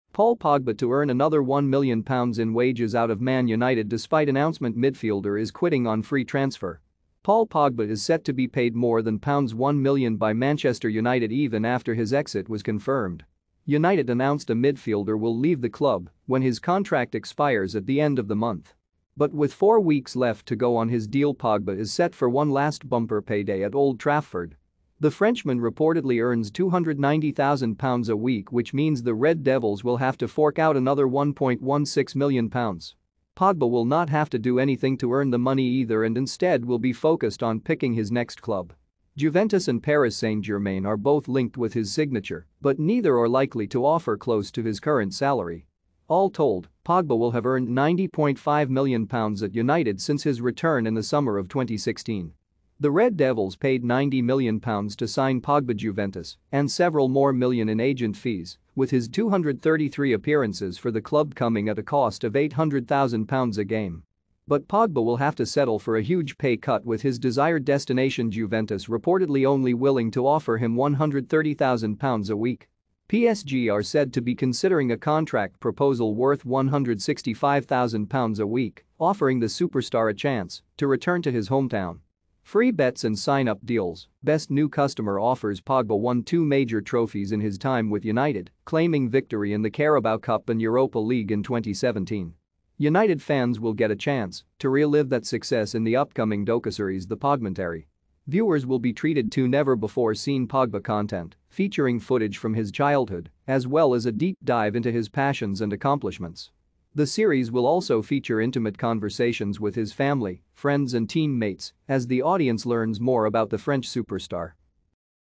Paul Pogba to earn another £1m in wages out of Man Utd despite announc
Paul Pogba to earn another £1m in wages out of Man Utd despite announcement midfielder is quitting on free transfer